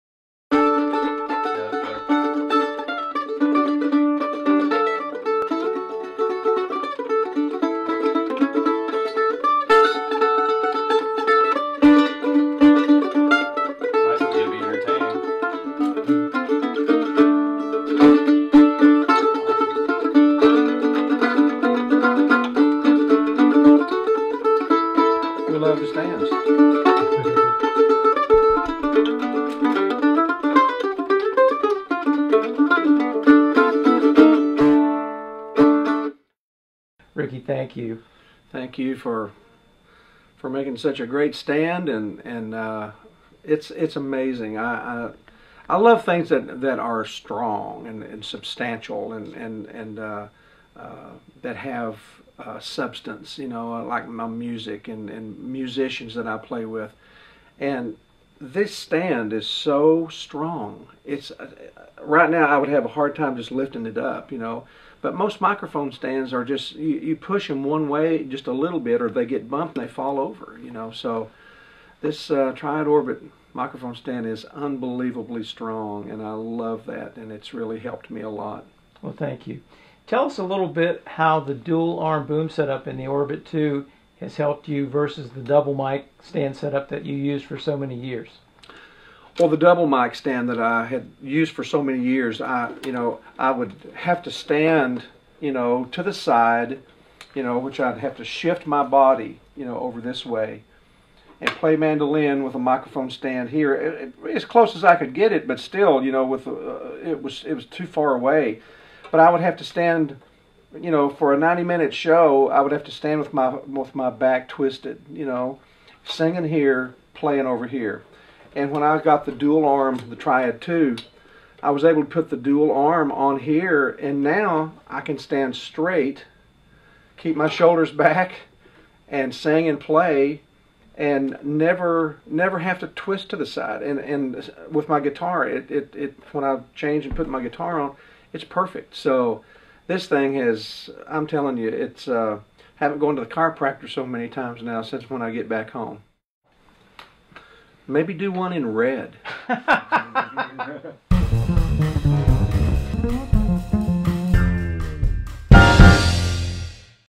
Ricky Skaggs talks to Triad-Orbit about how his Triad T3 stand and Orbit 2 / O2 boom.
The amazing Ricky Skaggs, country and bluegrass singer, musician, producer, and composer, talks to Triad-Orbit about how his Triad T2 stand and Orbit 2 / O2 boom has changed his life and set up. He primarily plays mandolin; however, he also plays fiddle, guitar, mandocaster and banjo.
The Orbit 2 with it's dual orbital booms make positioning his mics fast, easy and precise so he can perform at his best.
He now takes them on every show and tour.